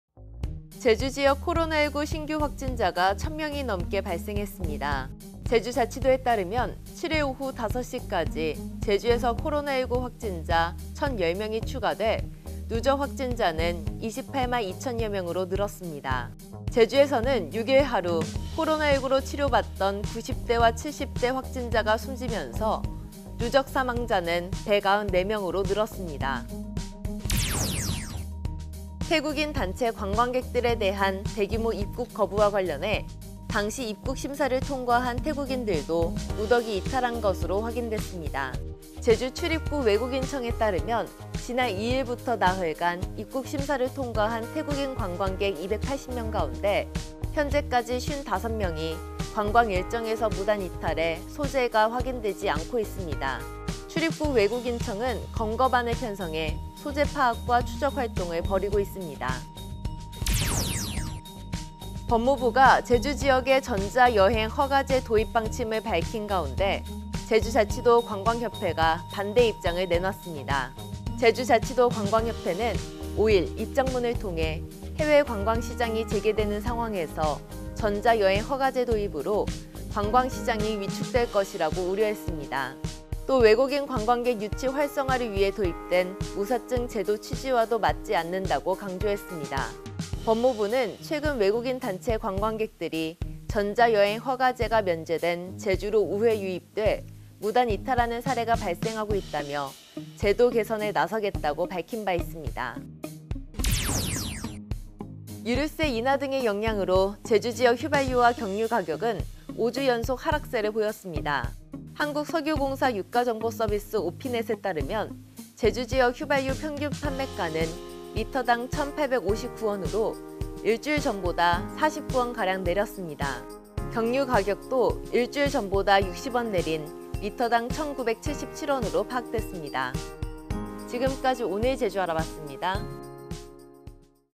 [📰] 제주관광협회 "전자여행허가제 도입 반대"｜｜제주 기름값 5주 연속 하락세｜220808｜지금제주는｜오늘의 제주 소식
[ LGU+ 185번 / olleh tv 162번 / SK브로드밴드 225번 / 스카이라이프 192번 ] 에서 만나 보세요